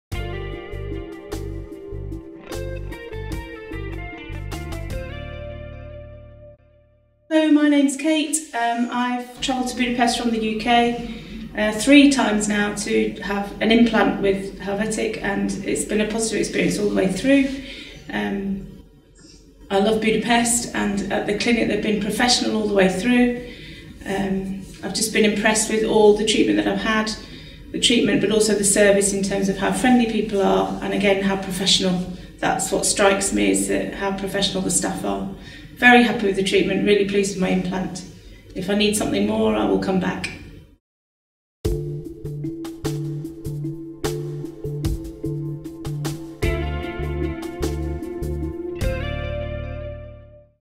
Extraction, implantation, root canal treatment, crowns - Helvetic Clinics Budapest Hungary Review
Reviews and testimonials Helvetic clinics Hungary, dental clinic and dentists in Budapest
Helvetic Clinics, your dental clinic in Budapest Hungary

(Click here for more details!)